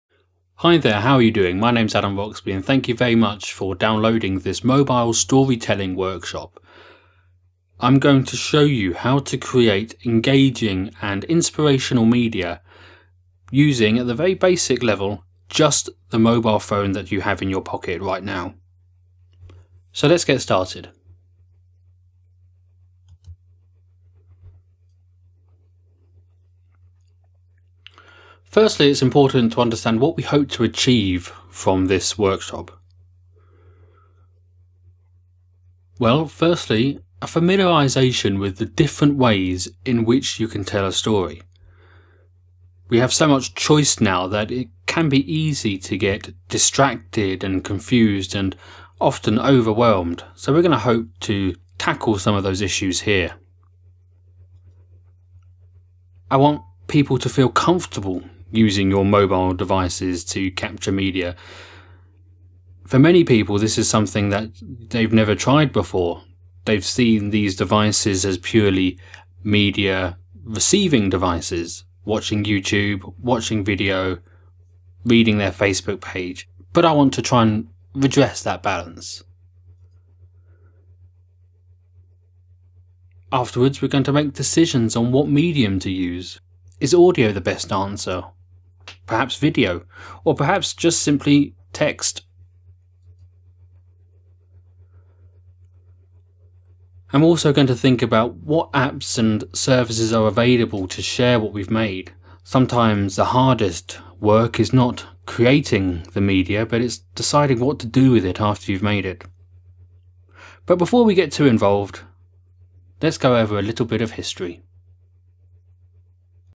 Mobile Story Telling Workshop - Teaser
A teaser for my upcoming 1 hour training on mobile story telling.